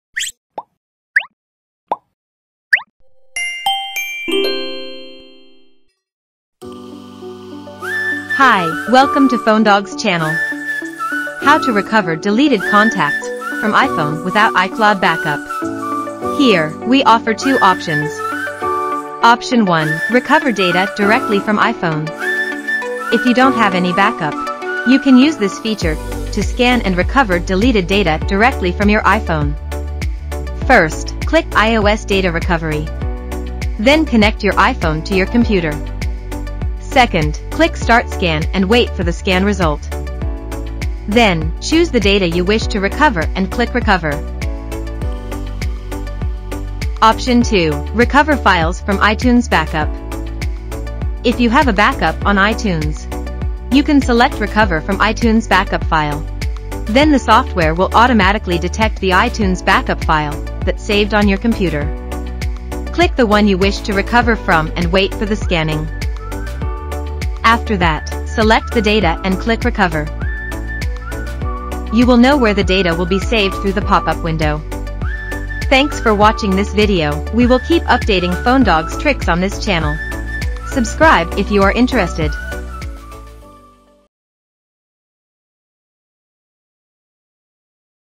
How to Recover Deleted Contacts from iPhone without iCloud Backup [Two Effective Methods]
How to Recover Deleted Contacts from iPhone without iCloud Backup? Here is a quick tutorial to show you 2 effective ways to recover deleted contacts from iPhone without iCloud backup. You can read this detailed instruction: ​

A bunch of reasons could lead to iPhone contacts missing, such as accidental deletion, update errors, iTunes restore errors, and more.

If your iPhone contact suddenly disappeared or you accidentally delete them, then this video will help you out with a chance to restore contacts on your iPhone quickly!

iCloud is designed to help users back up their contacts and other data, and restore their data when they accidentally lose it. Therefore, it would be better to back up your iPhone data regularly in case you lose important data, but if you don’t have any backup, don’t panic!
Why not try the methods in this video to increase your chance of getting your contacts back? We’re going to start with the most effective solutions to restore deleted contacts without any backups and another way to retrieve contacts from iPhone without iCloud.
The steps are simple to follow, come and try it now!
Way 1. Recover Deleted Contacts without Backup [Directly from iPhone]
Although it’s not an easy thing to find the deleted contacts back if there’s no backup, a professional tool - FoneDog iOS Data Recovery, could help a lot to automatically scan the iPhone device to search for the deleted data or files, and recover them easily.
Steps:
First, click iOS Data Recovery. Then connect your iPhone to your computer.
Second, click Start Scan and wait for the scan result.
Then, choose the data you wish to recover and click Recover.
Way 2. Recover Deleted Contacts without iCloud Backup [from iTunes Backup]
If you don’t have iCloud backup but do have iTunes backup, FoneDog could also detect your iTunes backup from your computer and restore contacts in minutes.
Steps:
Click the one you wish to recover from and wait for the scanning.
After that, select the data and click Recover.
You will know where the data will be saved through the popup window.

These two ways will free you from worrying about losing contact and other data. Give it a shot and find your contacts back in a more professional way.